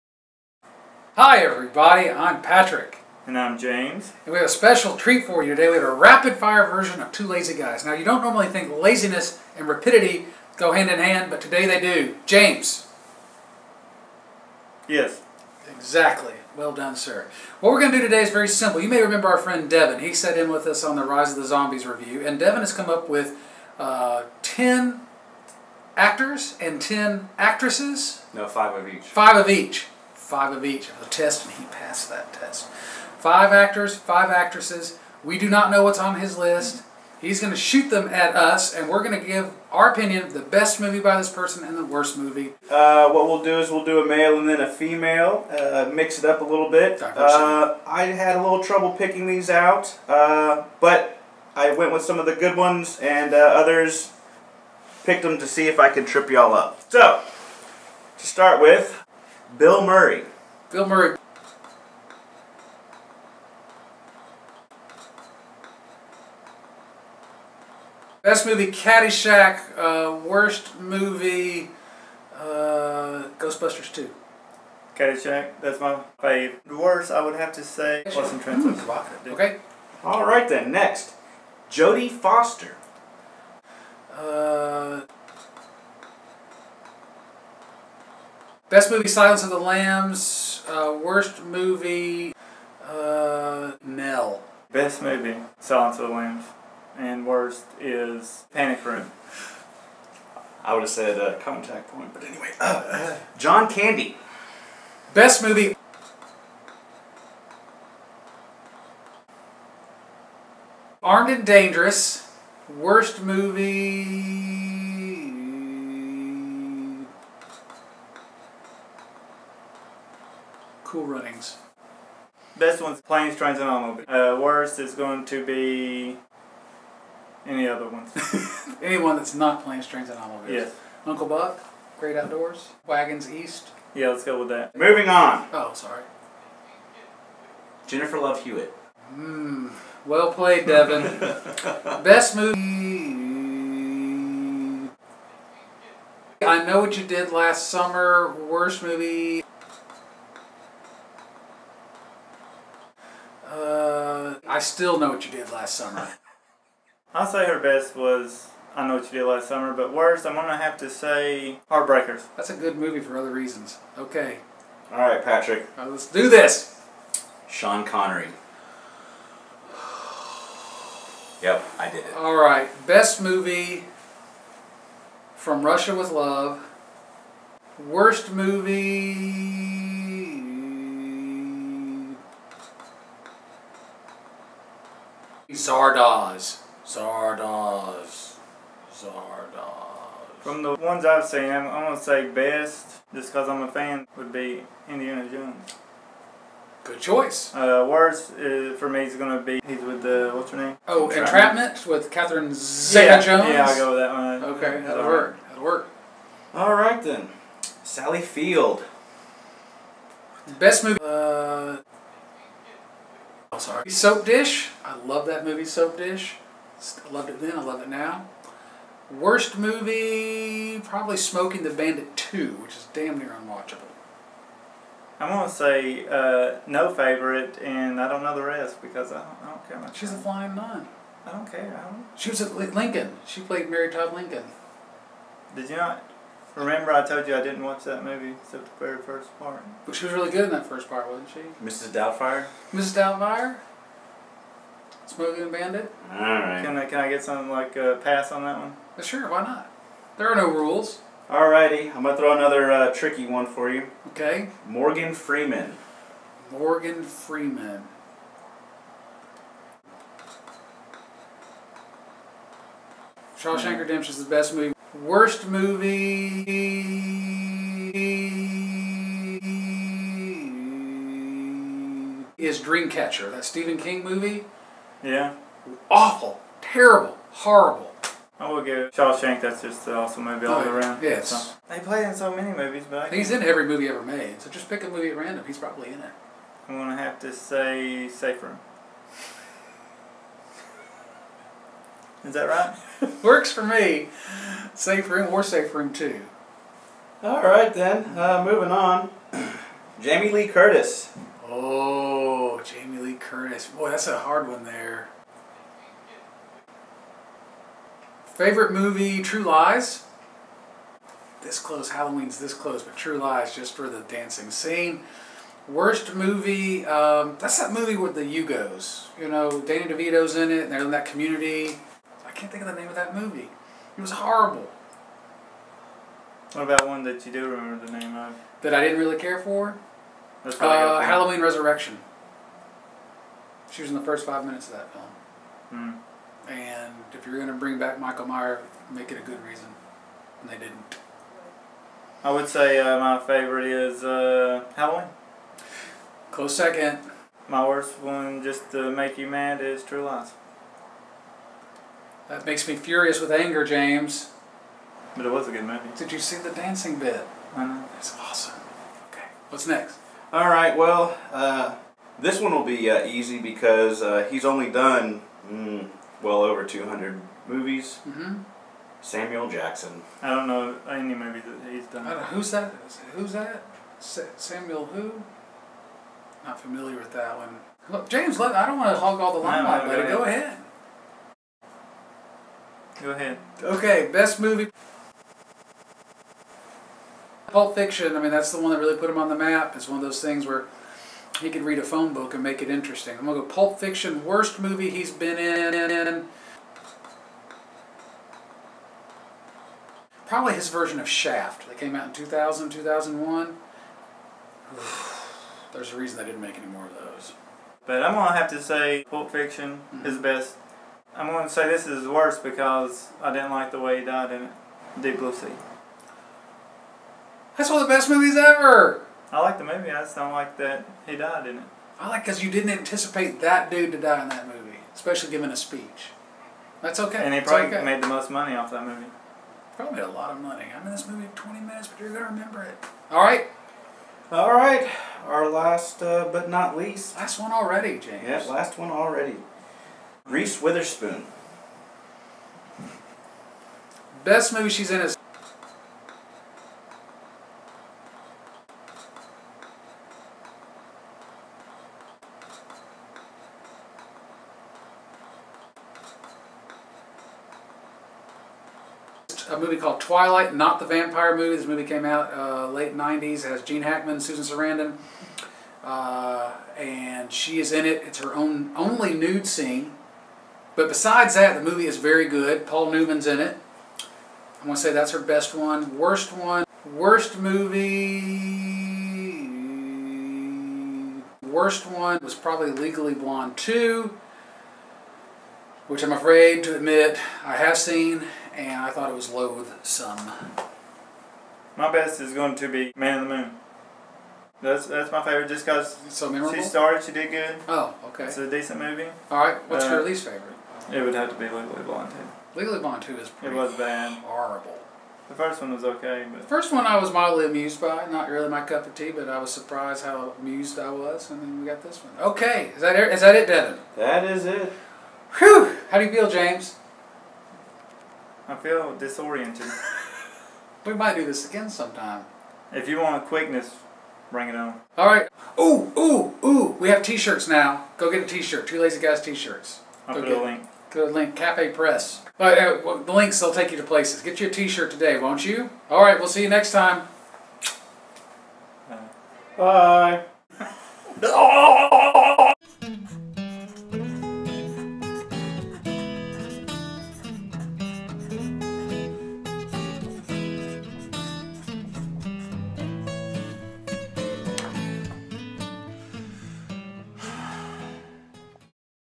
Rapid-fire! Five actors and five actresses we tell you there best movie and the worse!
Five actors and five actresses we tell you there best movie and the worse!